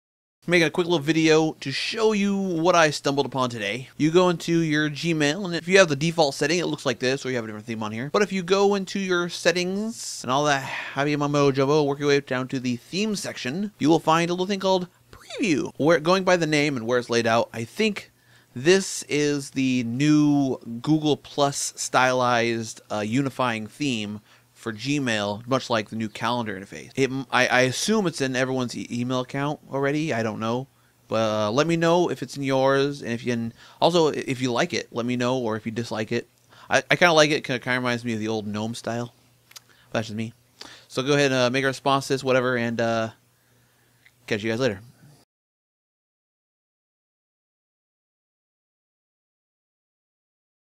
Quick Video: New Google+ Gmail Theme
The new theme they've snuck into Gmail.  It should be on everyone's Gmail account now.